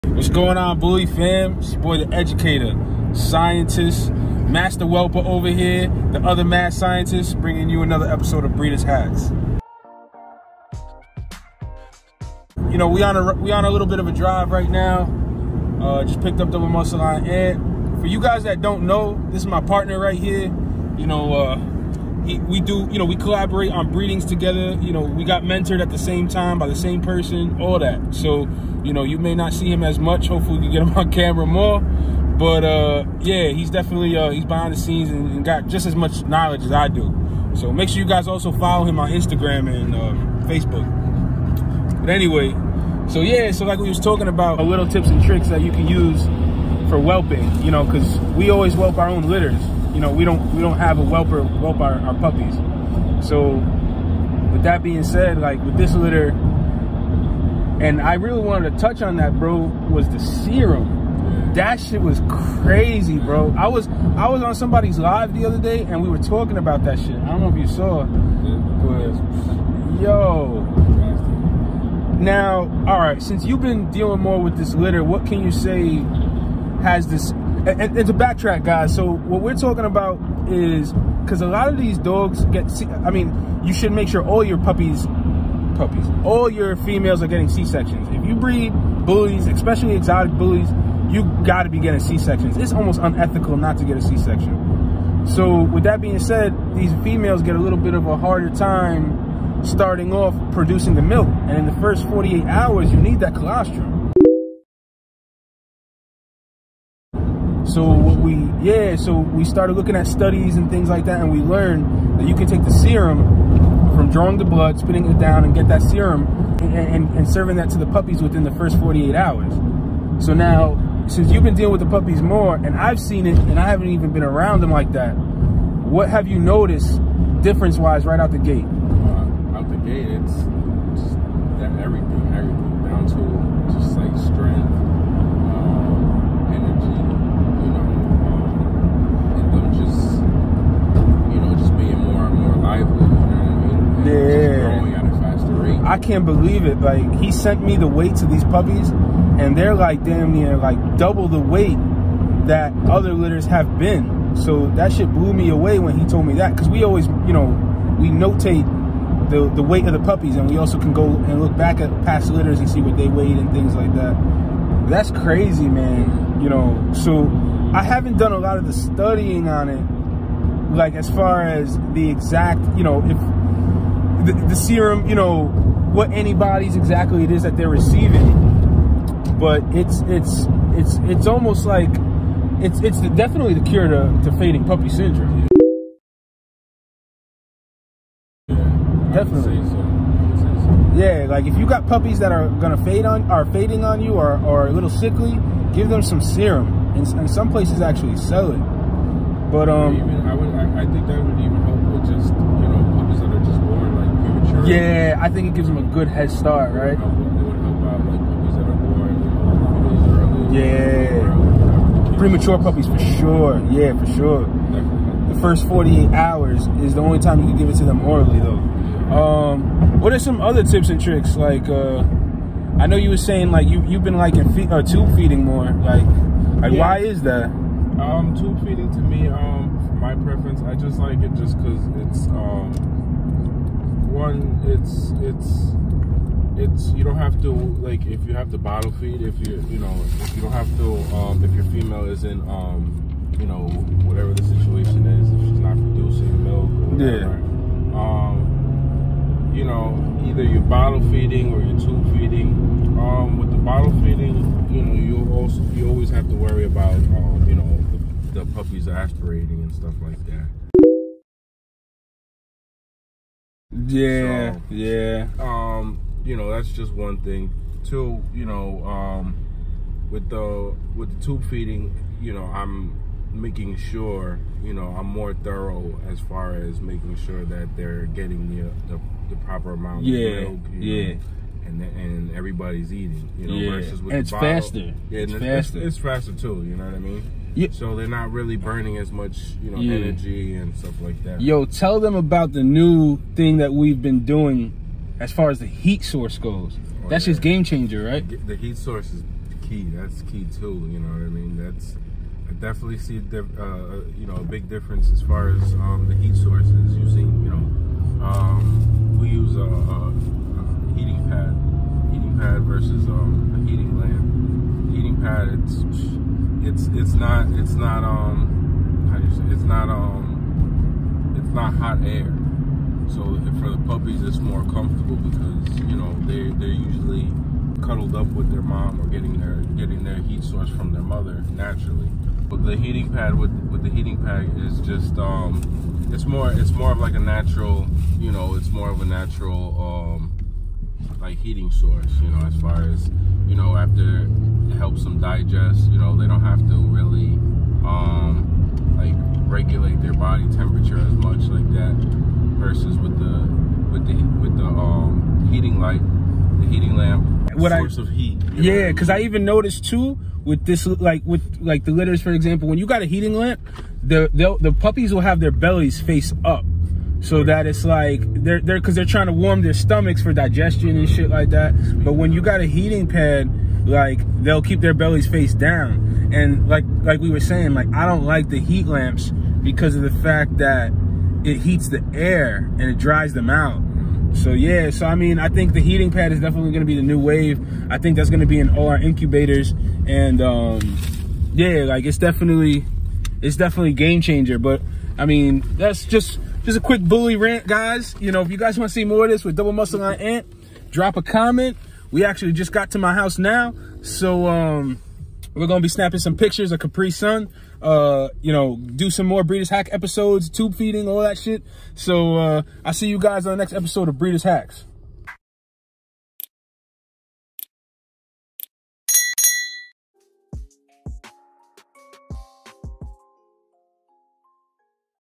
Bully Rant 📣:3/20/21 🗣🎙 Breeders Hacks‼️
Trying out a  NEW segment called “Bully Rants🎙🗣” where we sit down for some convo and just let the camera 🎥 just roll... Shout out to DoubleMuscleLineBulls-Ant for joining us, make sure to follow him.
If you would like to see more videos like this. Drop a Thumbs up👍